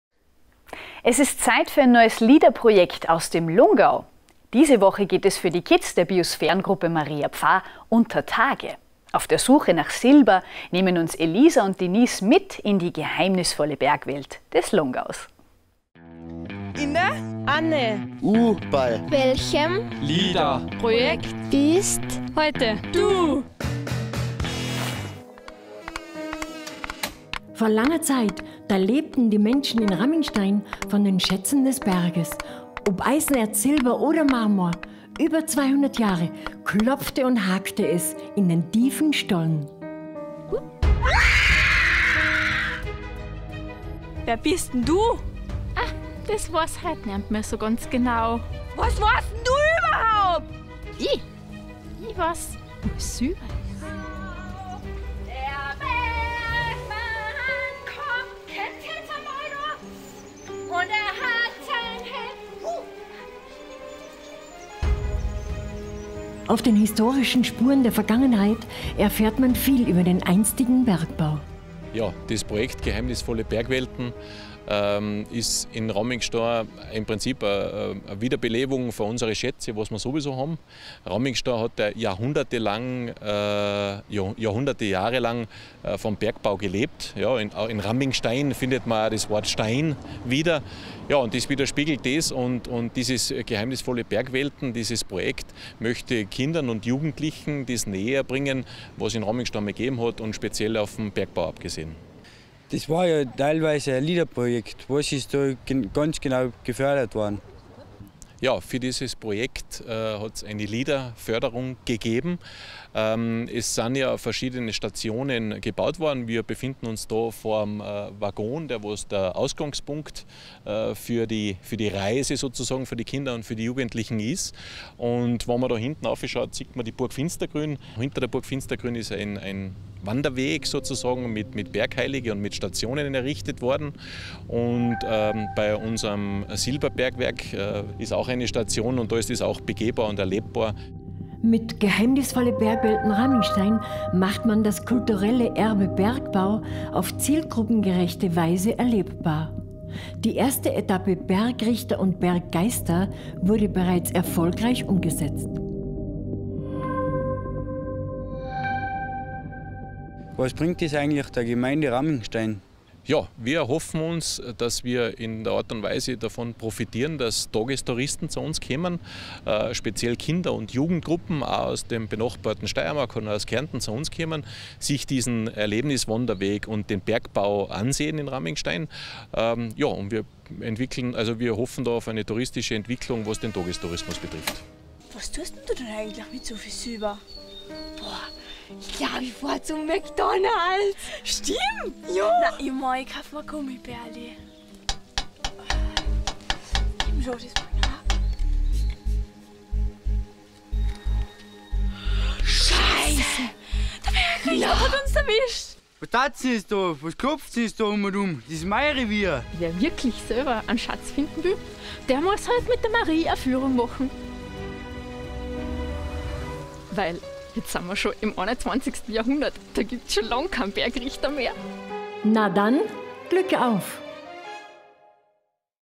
Projekt "Geheimnisvolle Bergwelten Ramingstein" - LEADER Biosphäre Lungau
Geheimnisvolle Bergwelten Ramingstein

Projektträger: Tourismusverband Tourismus Lungau Salzburger Land

Die verschiedenen touristischen Attraktionen im Zusammenhang mit dem Bergbau in Ramingstein wurden mit diesem Projekt in ein stimmiges und umfassendes touristisches Gesamtkonzept integriert, das vor allem Familien mit Kindern anspricht.

Der "Verein LEADER Biosphäre Lungau" unterstützt bei der Umsetzung von Projekten, die der nachhaltigen Entwicklung des Lungaus dienen.